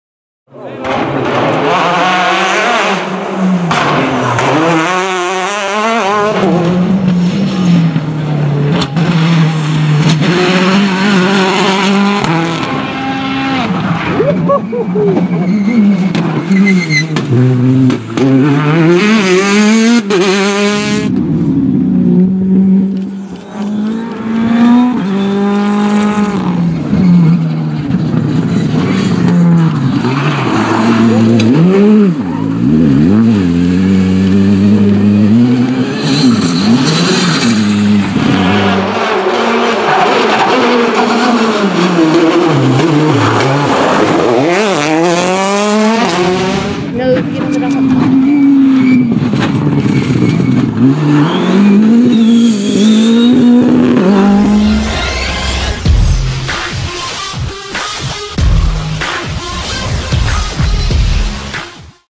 Rally de wallonie 2017 - ACTION & MISTAKSES [PREVIEW]
A preview of Rallye de Wallonie 2017 day 2.